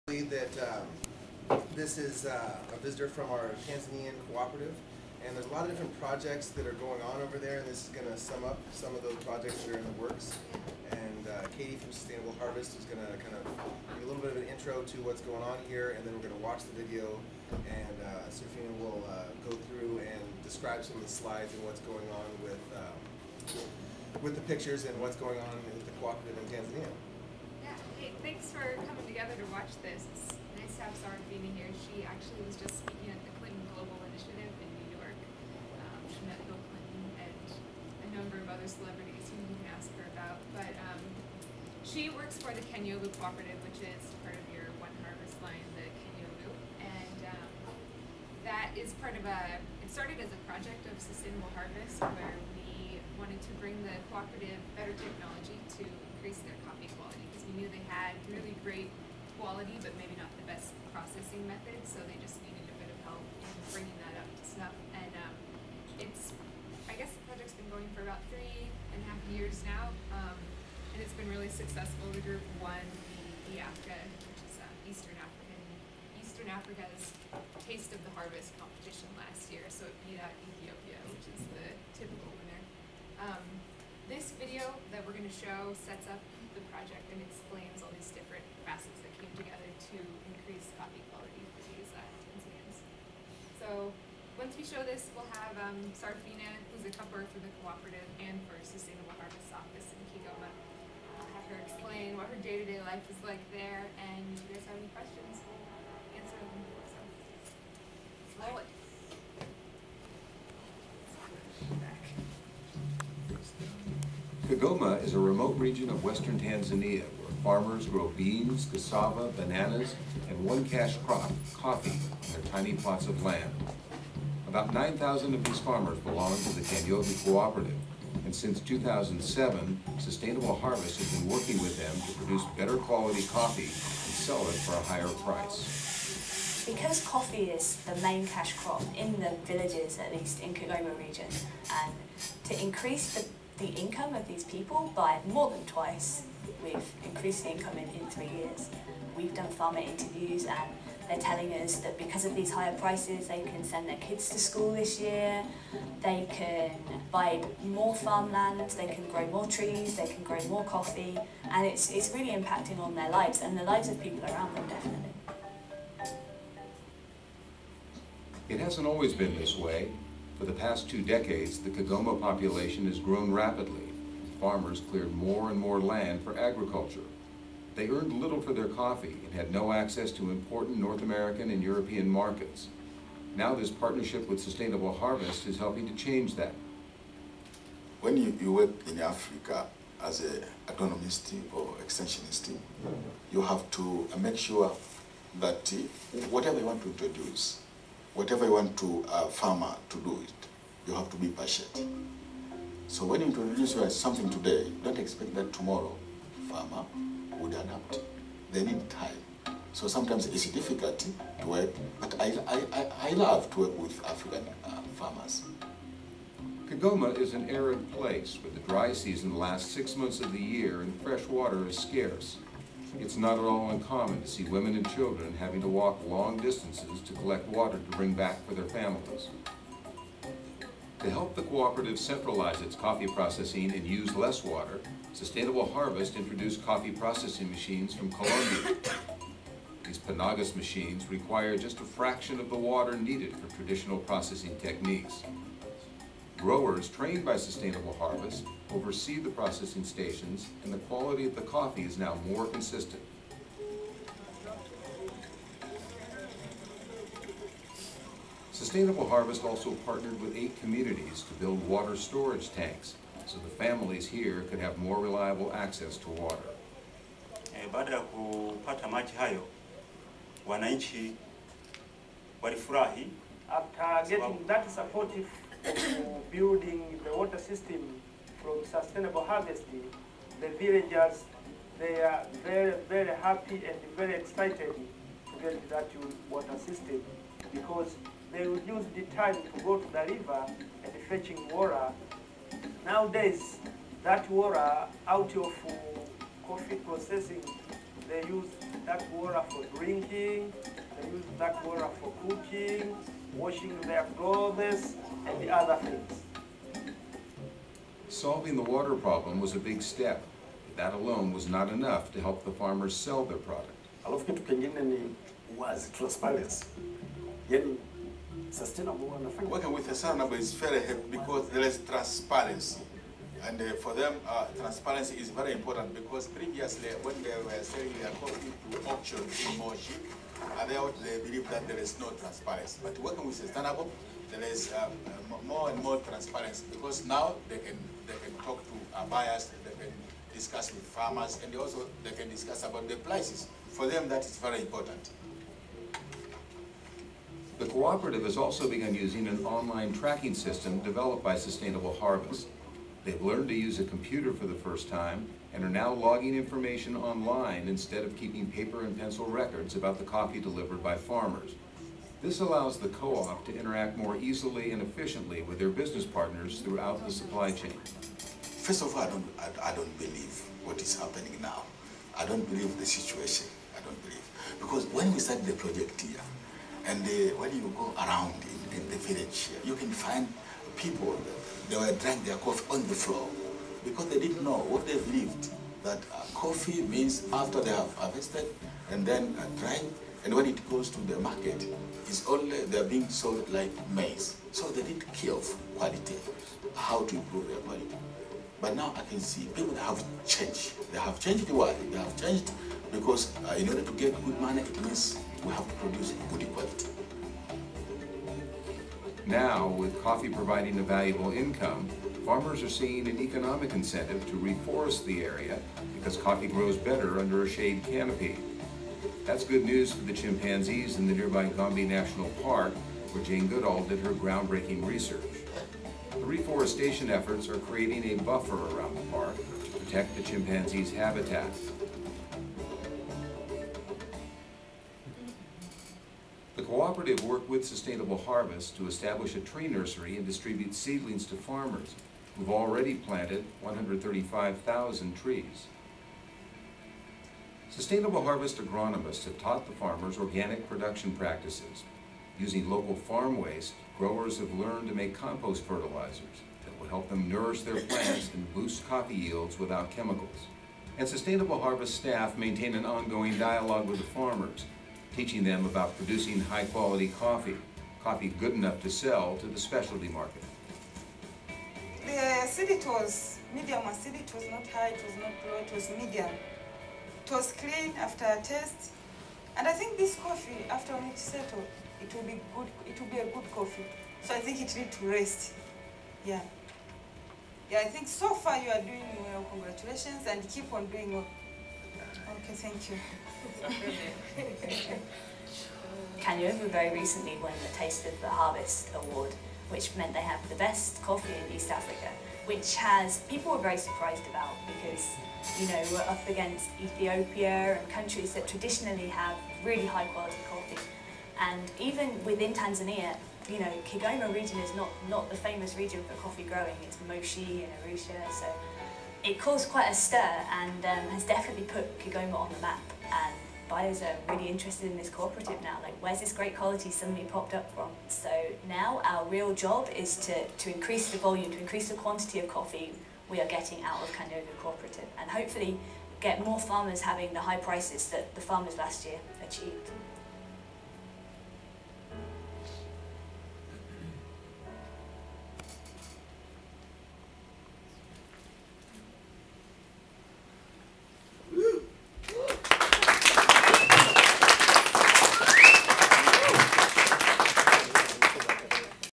Visit From the Kanyovu Cooperative to Dillanos Coffee Roasters
Dillanos was gifted to receive a special visit from a representative of the Kanyovu Cooperative and Sustainable Harvest. Giving us the chance to learn more about what is being done for their people and farms.